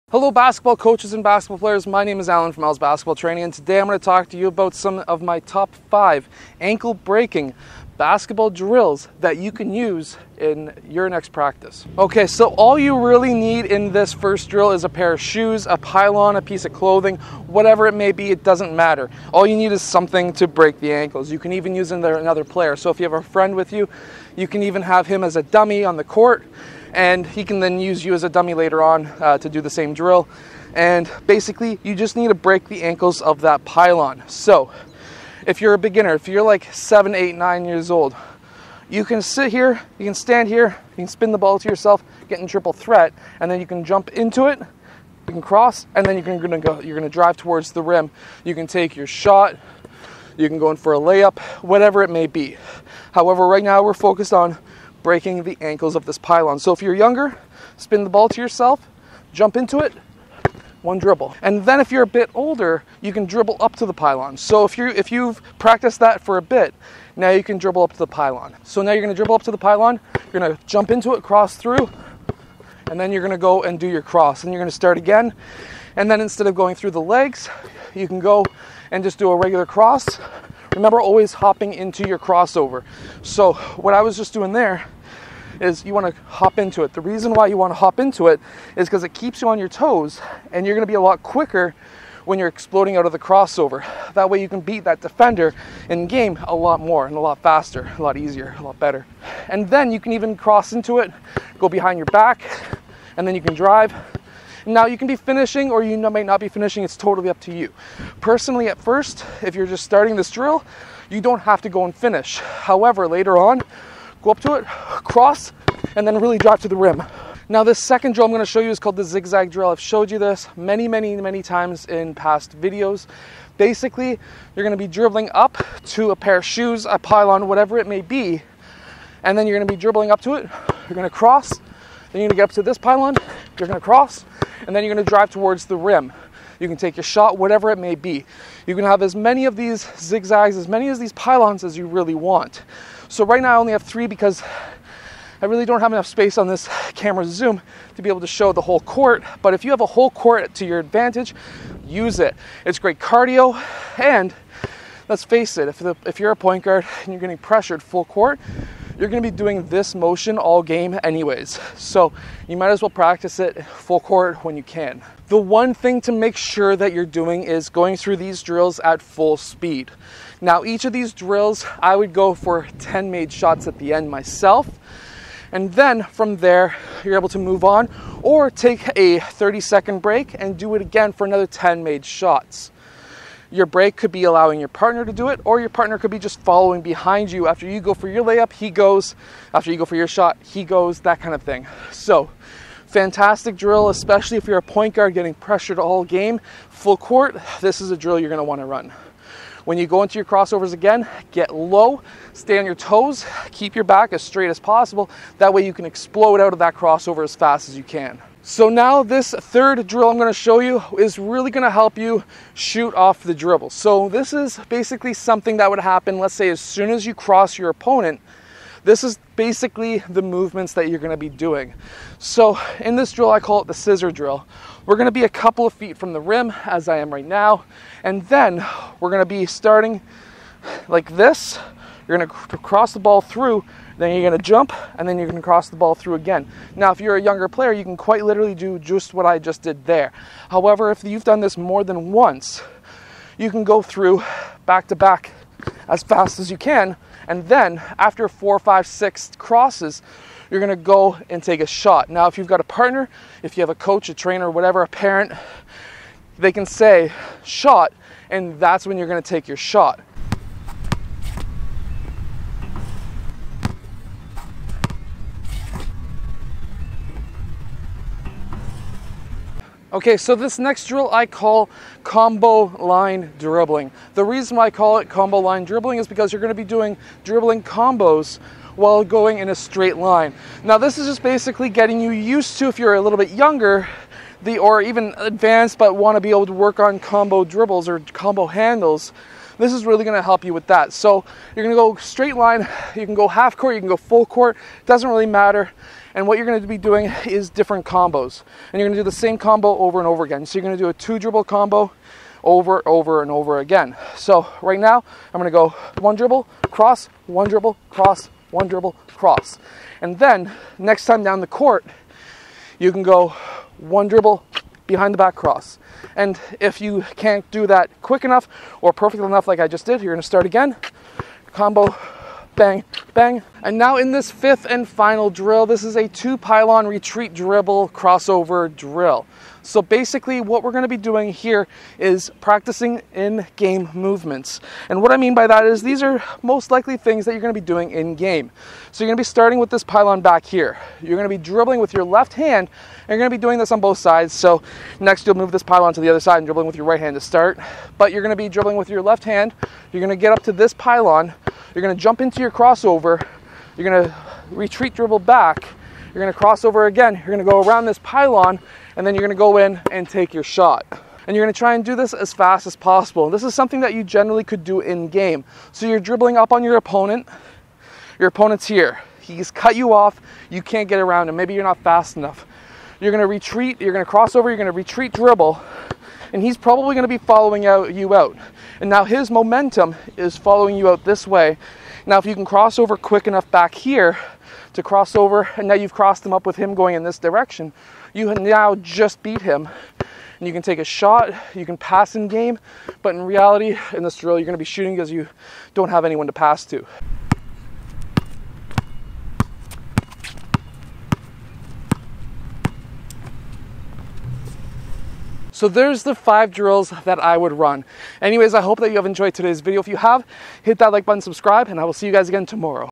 5 ANKLE BREAKING BASKETBALL DRILLS To Break More Ankles in Basketball
5 Ankle Breaking Basketball Drills To Break More Ankles in Basketball, in this video I breakdown 5 different Basketball dribbling drills that can help you break more ankles in Basketball. These drills will help you with your crossovers, dribbling, shooting off the dribble and shooting off the dribble after breaking someones ankles.

Amazon links
The Basketball I use

1 on 1 Basketball Training (Hamilton Ontario and Surrounding Areas)